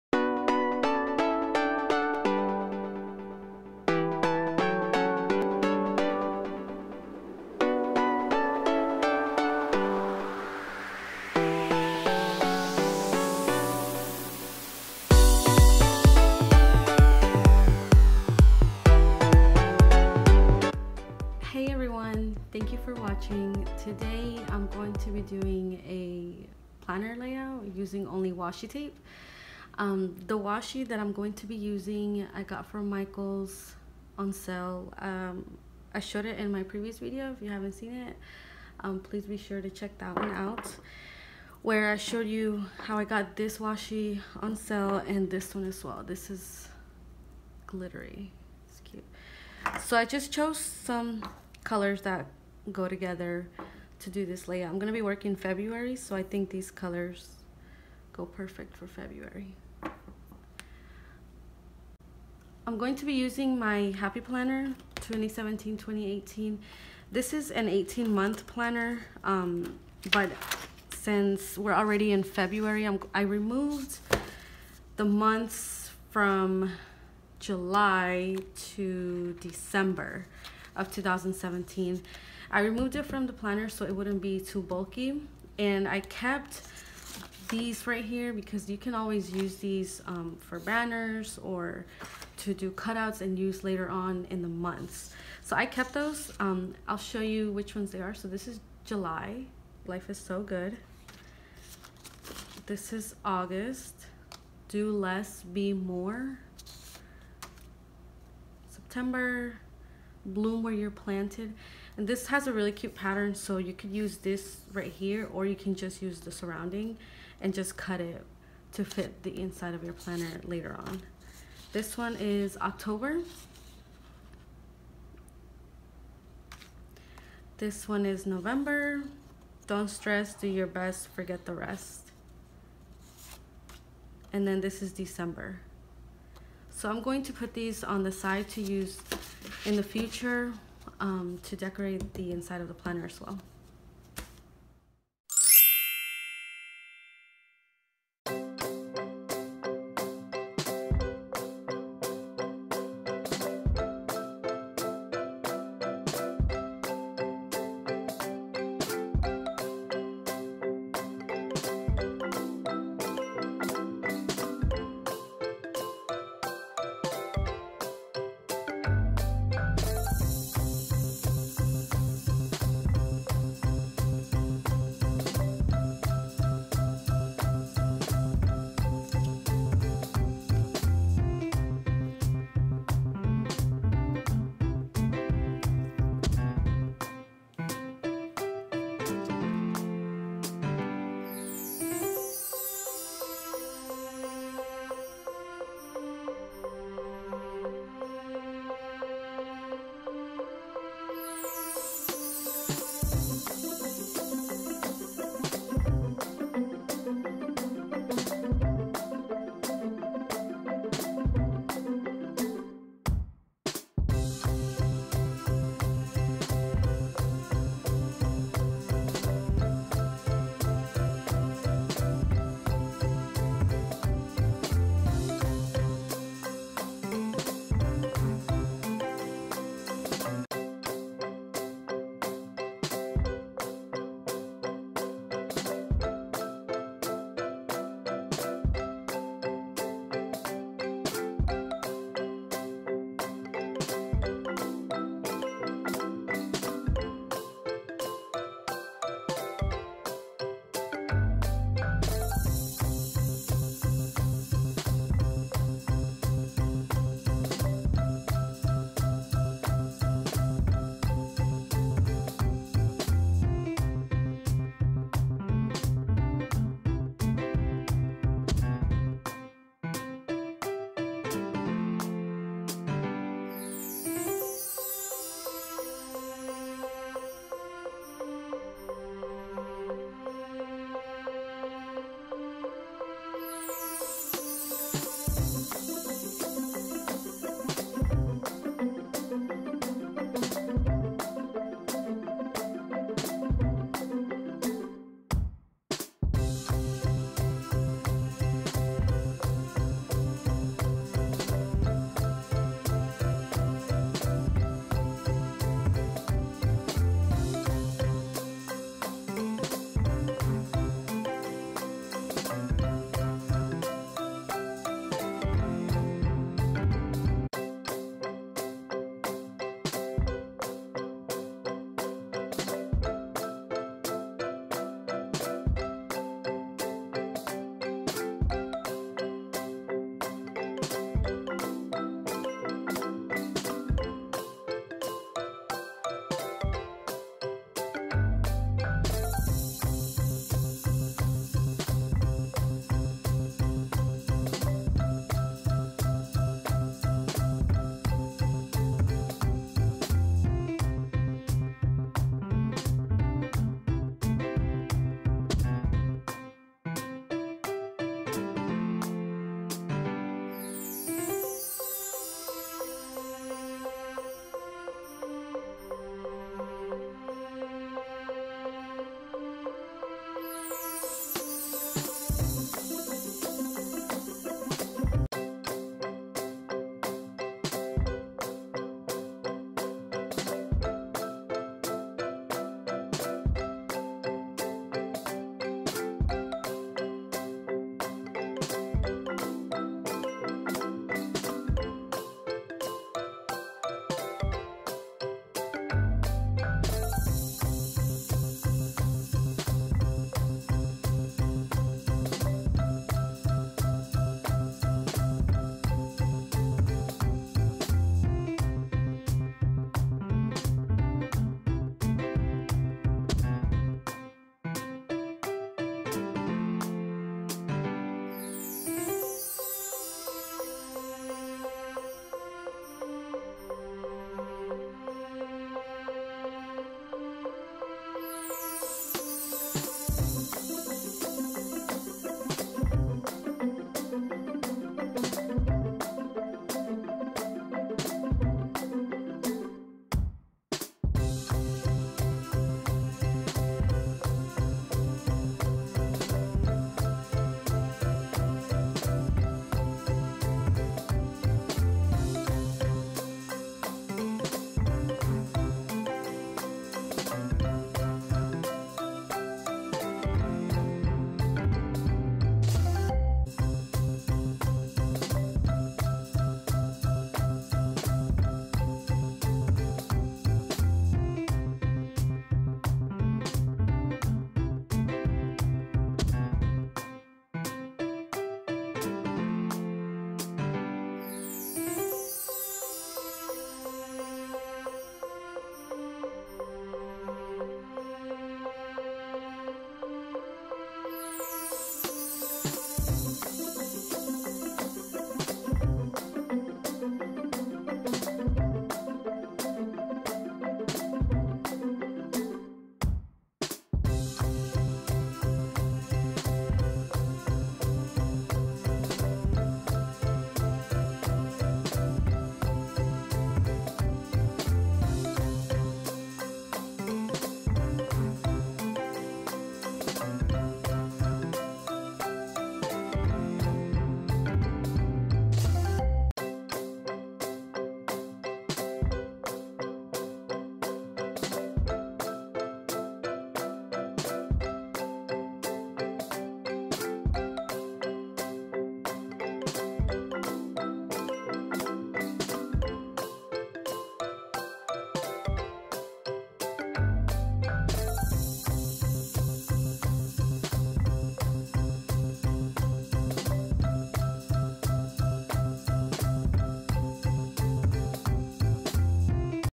PLAN WITH ME USING ONLY WASHI! // Happy Planner
PLAN WITH ME USING WASHI ONLY

Don't have sticker sets to decorate your planner? In this video I'll show you how to decorate your planner using only washi tape!